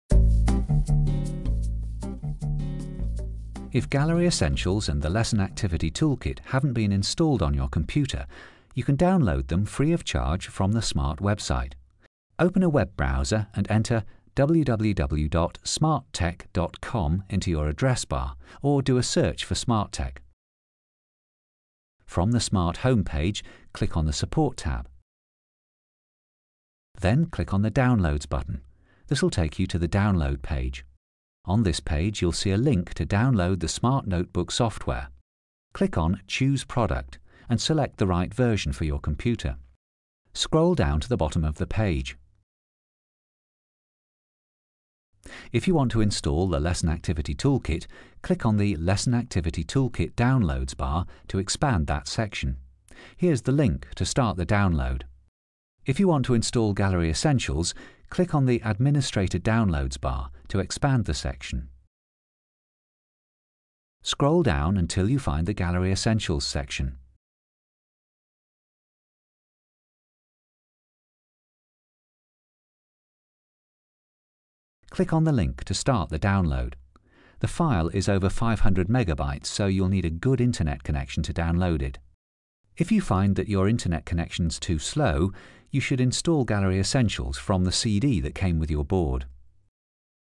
SMART Notebook - Install Gallery essentials
This tutorial video helps you download and install gallery essentials.